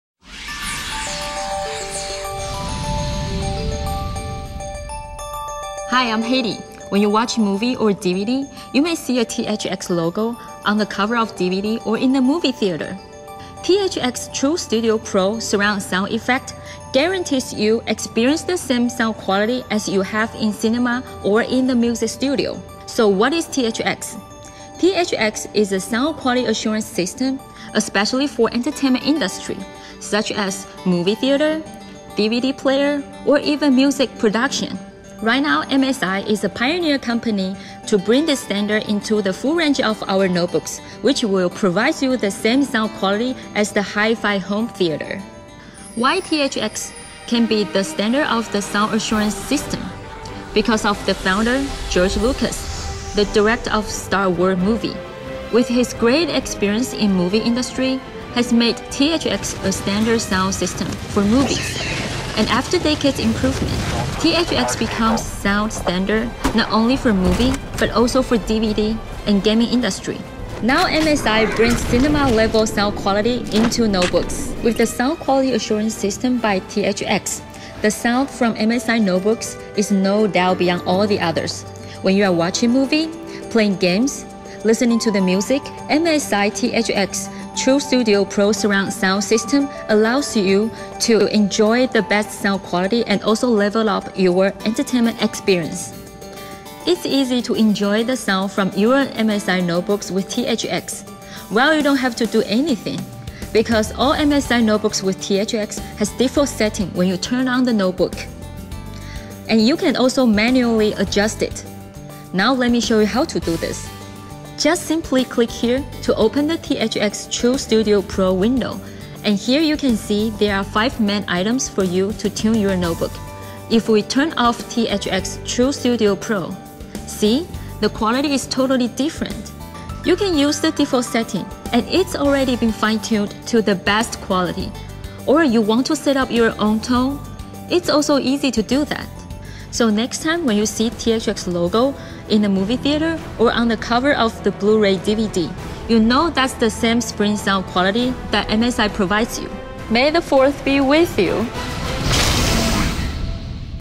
Estándar de sonido THX en las notebooks MSI
THX es un estándar de calidad de sonido usado inicialmente en la industria cinematográfica, que se ha perfeccionado y extendido a la industria de gaming, producciones musicales y DVDs.  Esta es una de las características que usted puede encontrar en las notebooks MSI.